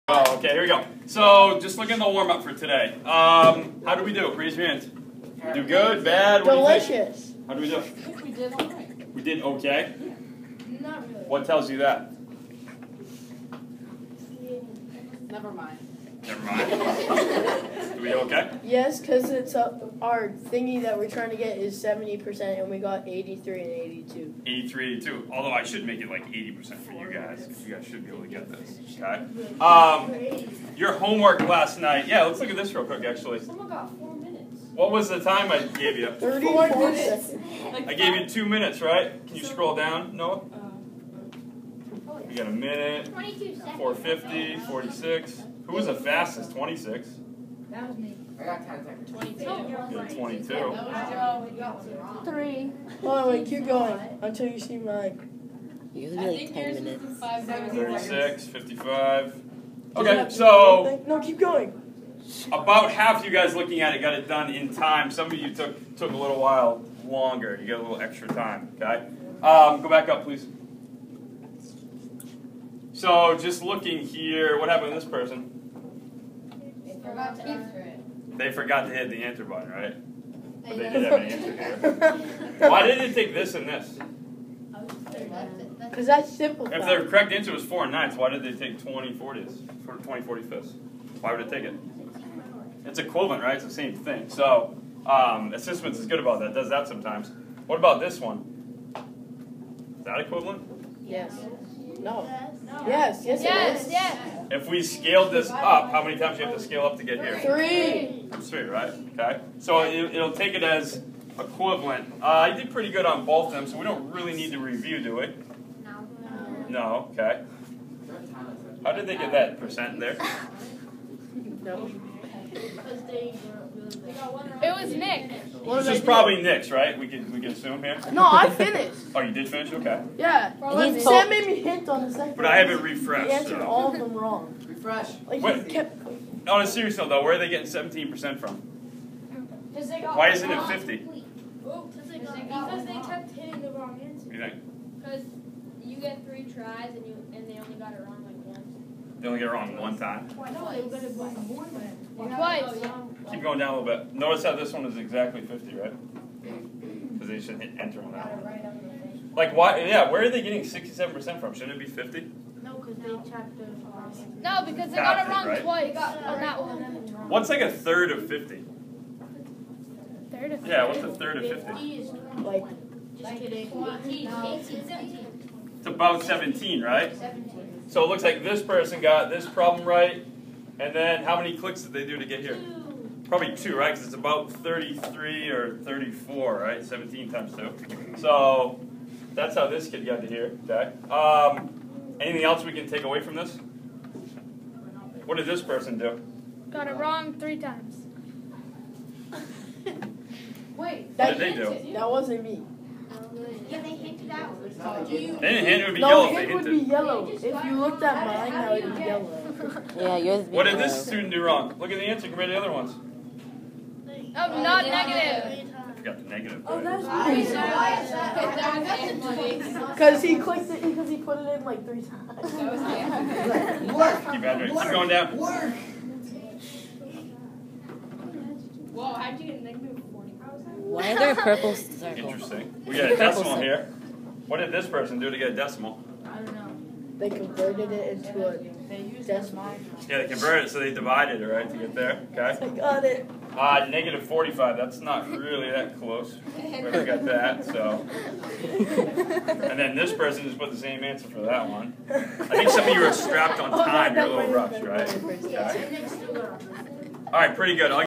Classroom demo: Warmups with ASSISTments
Mr. Broaddus is a middle school math teacher. See how Mr. Broaddus gets his students warmed up for class with ASSISTments.